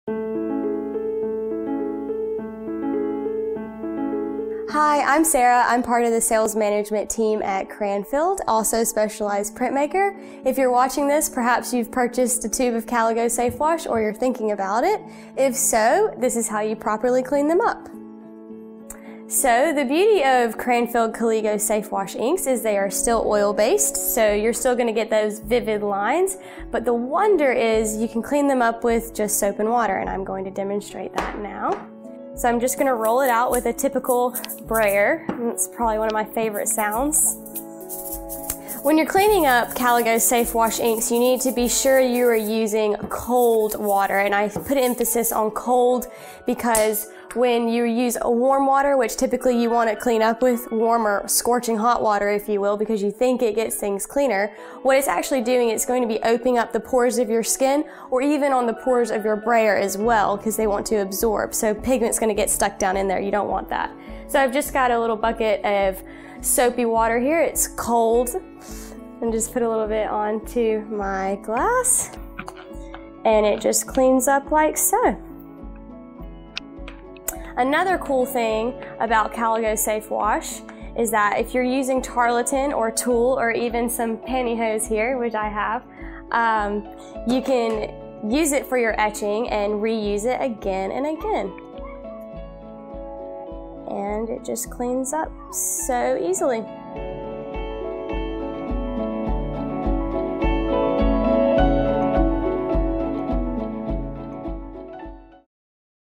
Caligo Clean Up
Our Cranfield Colours US ambassador, Sarah Carnline, provides helpful tips for cleaning up our Caligo Safe-Wash Inks.

to discover our Cranfield Colours Instgram, podcasts and website.